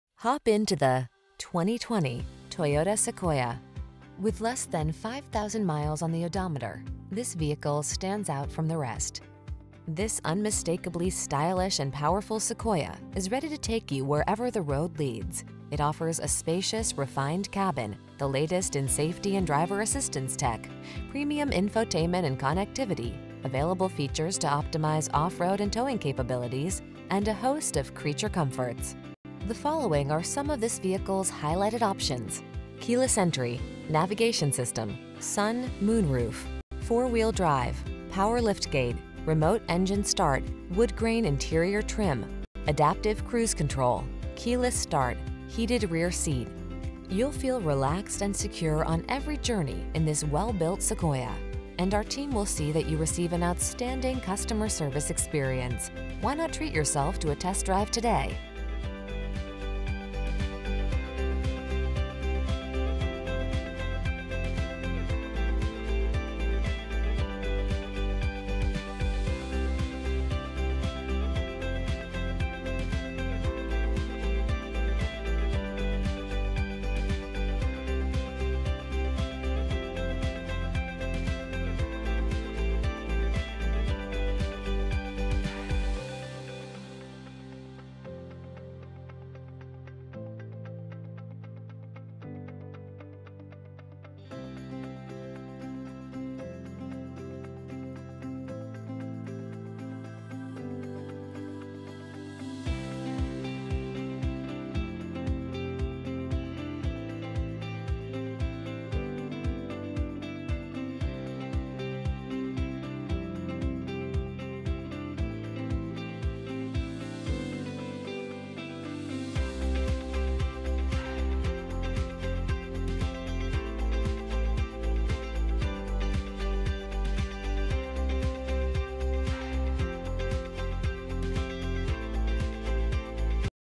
2020 Toyota Sequoia near me Elmhurst, Carol Stream, Bloomingdale, Itasca, Hinsdale, IL 10158A
White Used 2020 Toyota Sequoia available near me in Elmhurst, Illinois at Honda on Grand. Servicing the Carol Stream, Bloomingdale, Itasca, Hinsdale, IL area.

2020 Toyota Sequoia White CARFAX One-Owner. Clean CARFAX. Platinum 4WD 6-Speed Automatic 5.7L V8 CARFAX ONE OWNER!!, local trade in.Call or Come to Honda On Grand in Elmhurst and get a deal that you can smile about!